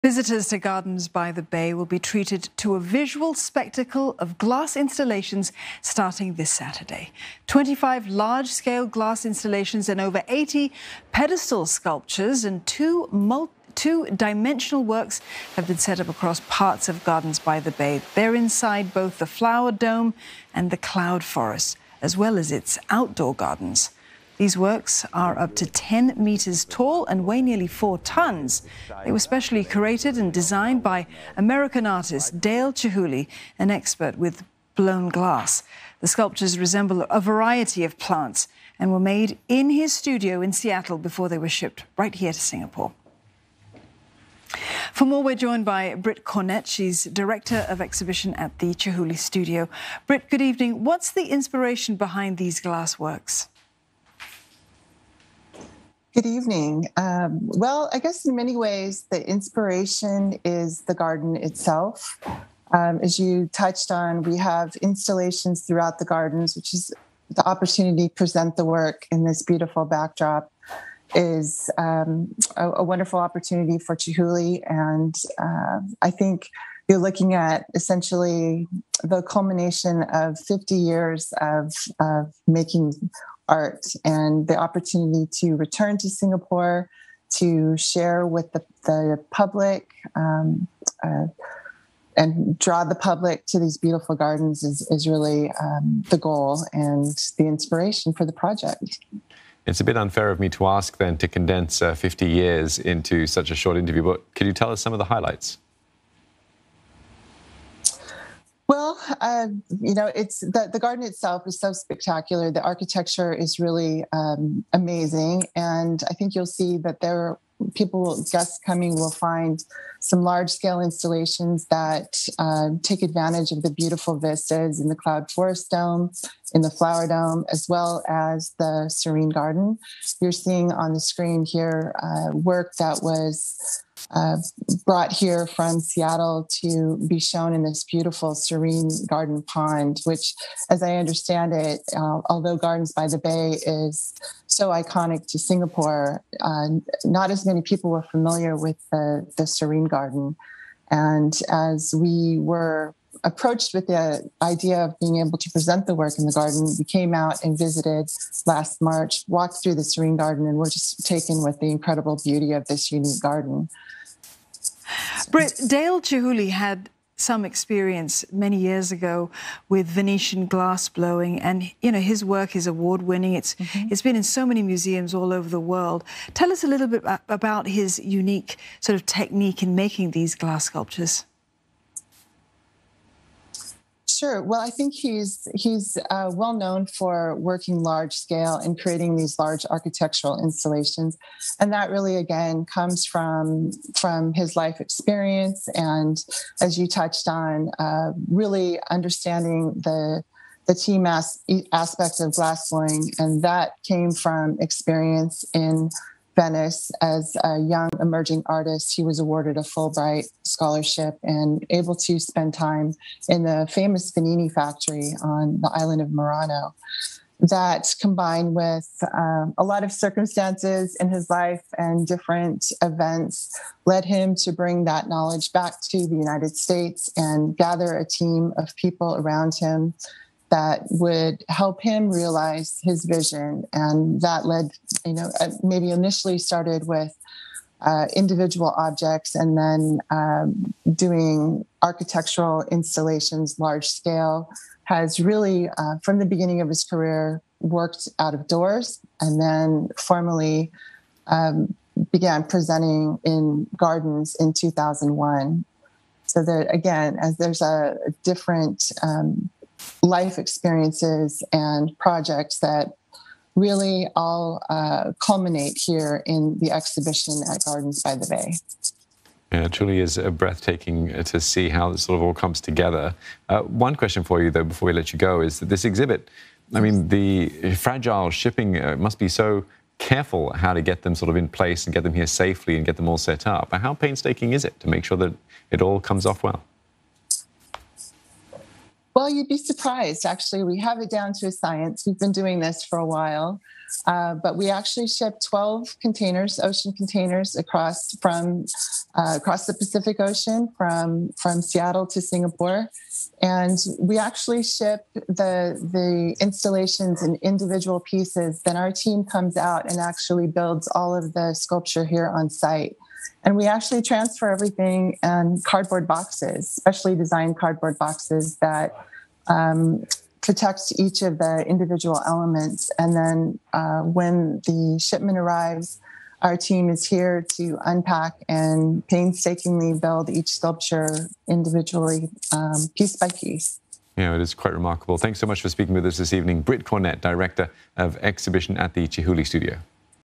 Glass installations on display at Gardens by the Bay from May 1
Visitors to Gardens by the Bay will be treated to a visual spectacle of glass installations from May 1. Twenty-five large-scale glass installations and more than 80 pedestal sculptures and two-dimensional works have been set up across parts of the attraction. Britt Cornett, Director of Exhibition at the Chihuly Studio, spoke about the inspiration behind the glass installations.